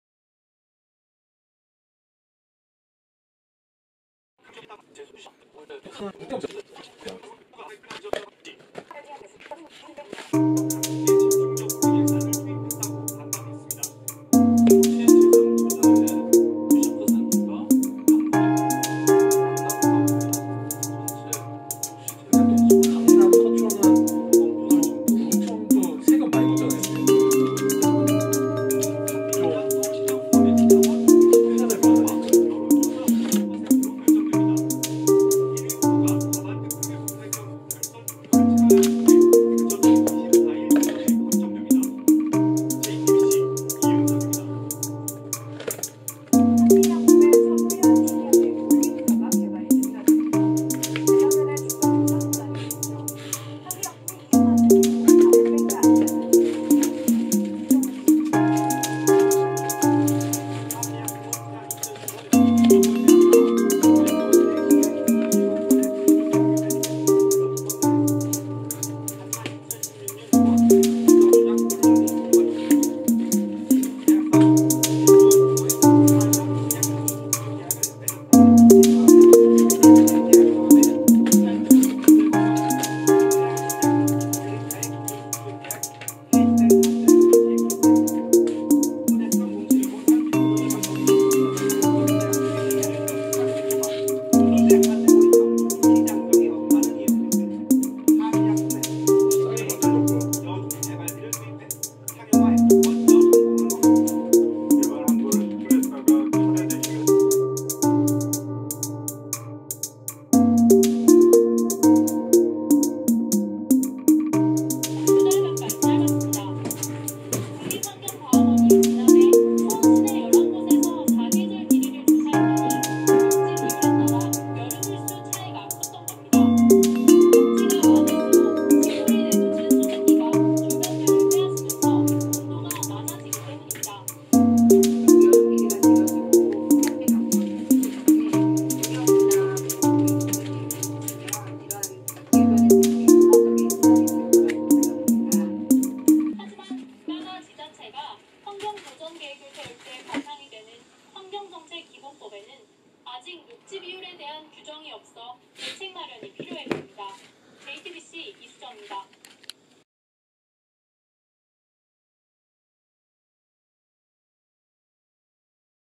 Sony voice Recorder SX2000
소니에서 나온 보이스 레코더 SX2000입니다.
깔끔하고 직구를 통하면 아주 저렴한(?)가격으로 구할 수 있는 레코더입니다.

무손실 녹음기능도 가지고 있는 훌륭한 녹음기 입니다.